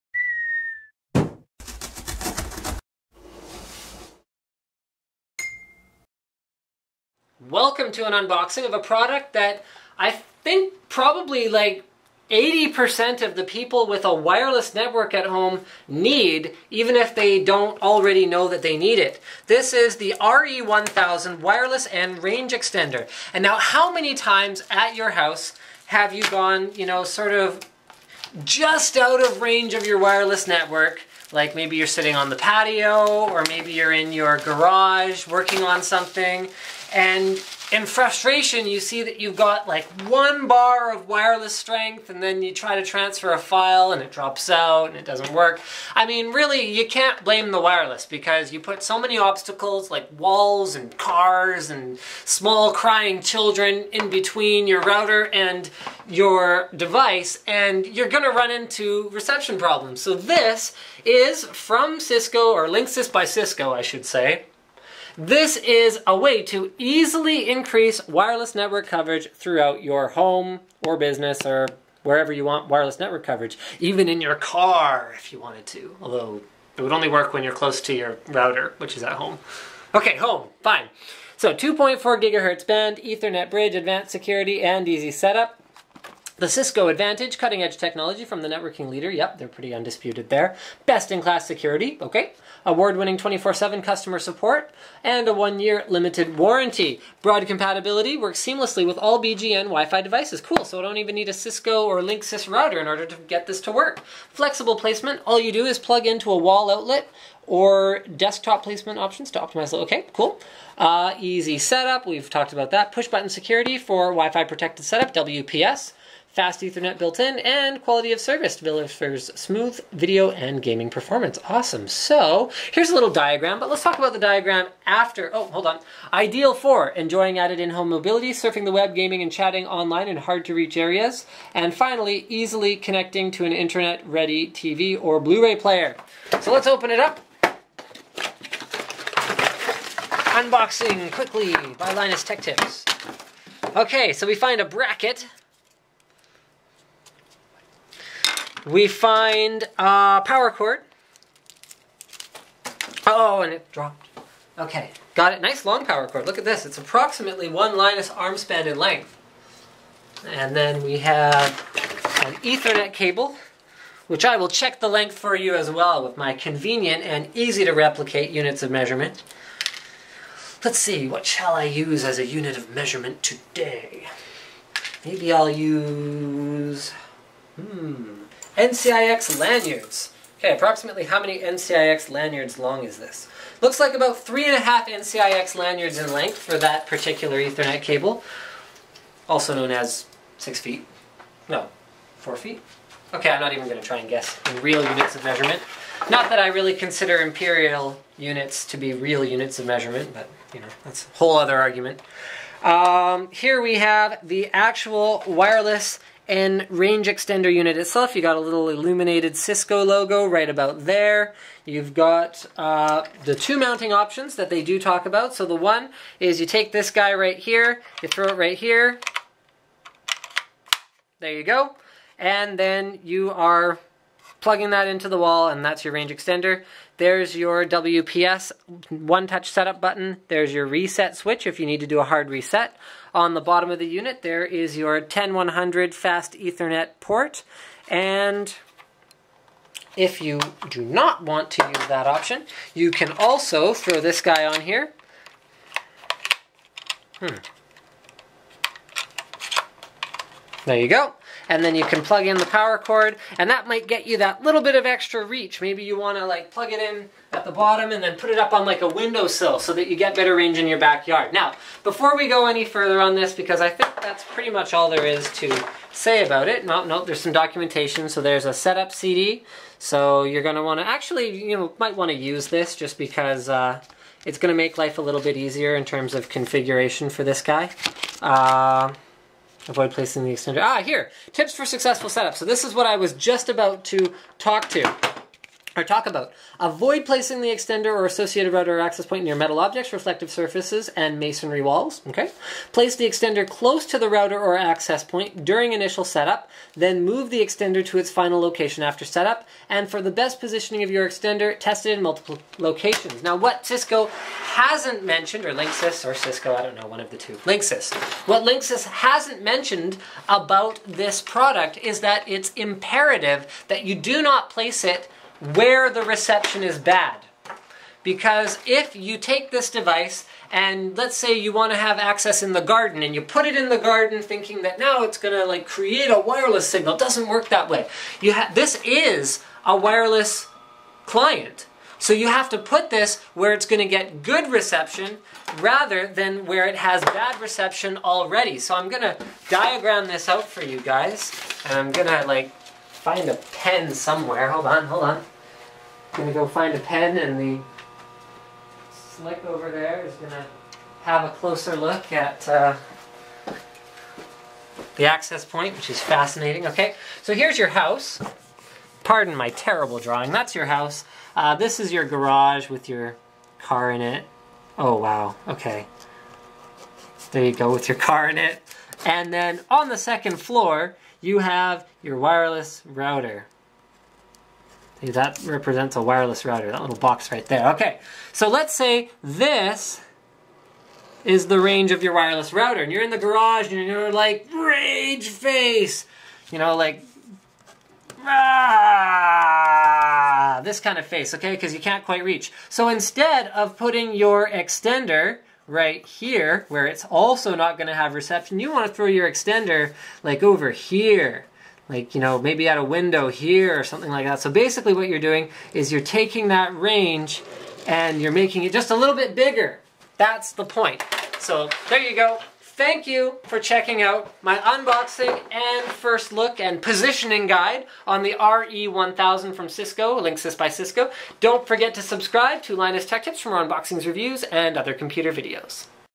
Linksys RE1000 Wireless N Range Extender Unboxing & First Look Linus Tech Tips
This thing looks really handy. Check out the unboxing to see it and some tips for how to deploy it!